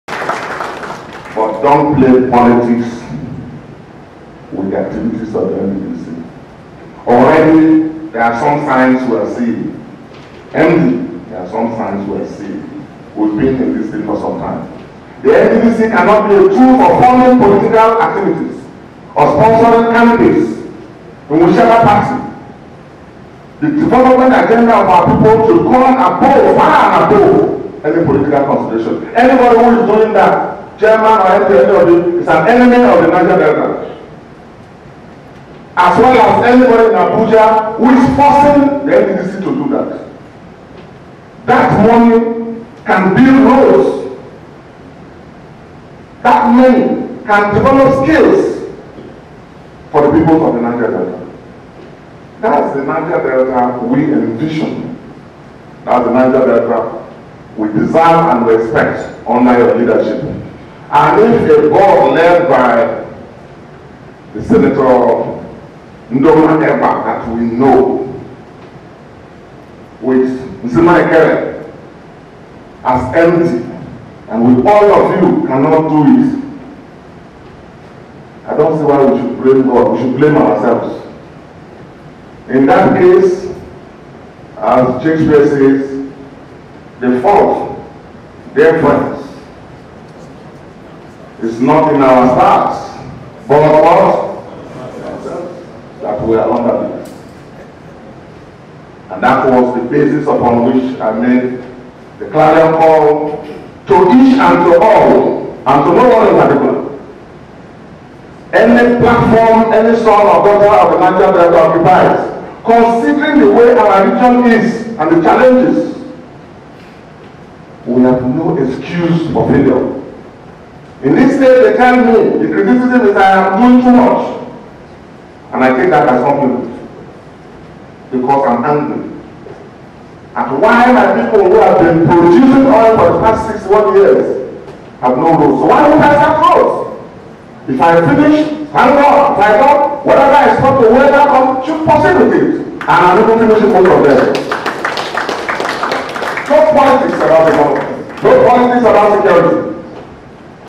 HSDickson - Bayelsa is Safe Gov. Dickson Speaks on Developement
There has been a total lack of developement in Bayelsas state. This is reflected in every sector in Education, Infrastructure, health, commerce etc. Governor Dickson speaks on why Bayelsa is in a hurry for development.